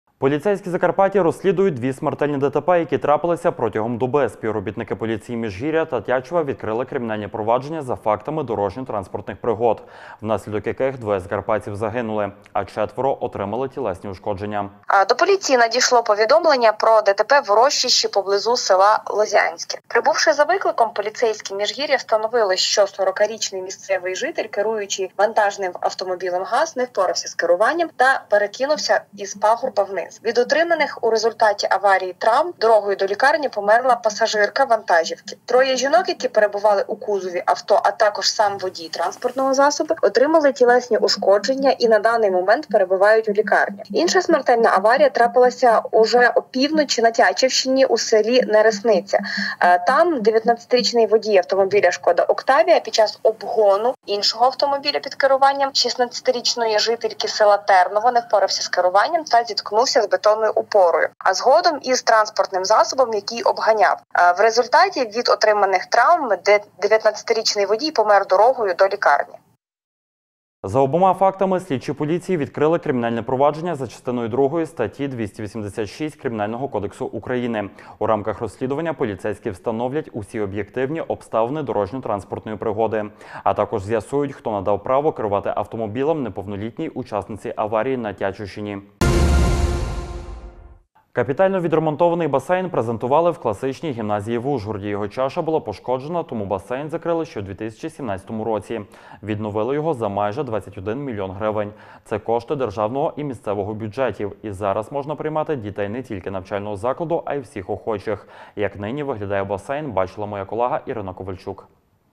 Смертельні ДТП за добу. Загинули люди.
Поліцейські Закарпаття розслідують дві смертельні ДТП, що трапилися протягом доби. Співробітники поліції Міжгір’я та Тячева відкрили кримінальні провадження за фактами дорожньо-транспортних пригод, внаслідок яких двоє закарпатців загинули, а четверо – отримали тілесні ушкодження.За обома фактами слідчі поліції відкрили кримінальні провадження за частиною 2 статті 286 Кримінального кодексу України. У рамках розслідування, поліцейські встановлять усі об’єктивні обставини дорожньо-транспортних пригод, а також з’ясують, хто надав право керувати автомобілем неповнолітній учасниці аварії на Тячівщині.